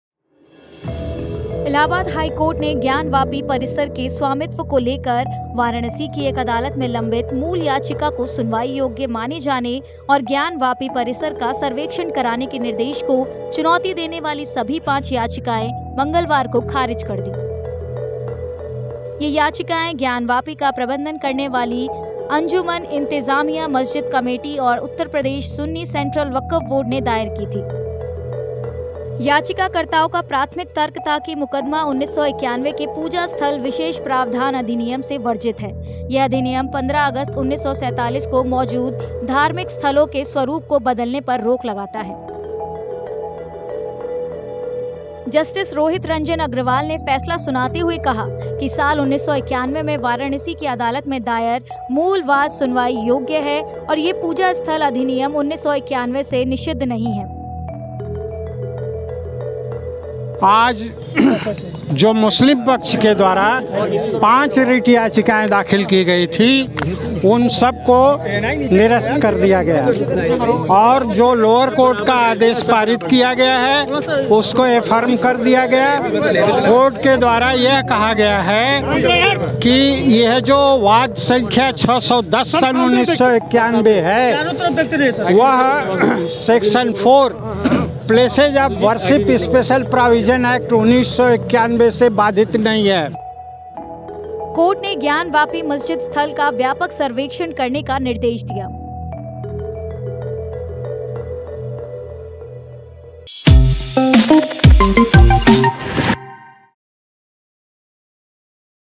Gyanvapi Case: इलाहाबाद हाई कोर्ट ने खारिज कीं ज्ञानवापी से जुड़ी मुस्लिम पक्ष की सभी 5 याचिकाएं
इलाहाबाद हाई कोर्ट ने ज्ञानवापी परिसर के स्वामित्व को लेकर वाराणसी की एक अदालत में लंबित मूल याचिका को सुनवाई योग्य माने जाने और ज्ञानवापी परिसर का सर्वेक्षण कराने के निर्देश को चुनौती देने वाली सभी पांच याचिकाएं मंगलवार को खारिज कर दीं।

ये याचिकाएं ज्ञानवापी का प्रबंधन करने वाली अंजुमन इंतेजामिया मस्जिद कमेटी और उत्तर प्रदेश सुन्नी सेंट्रल वक्फ बोर्ड ने दायर की थीं।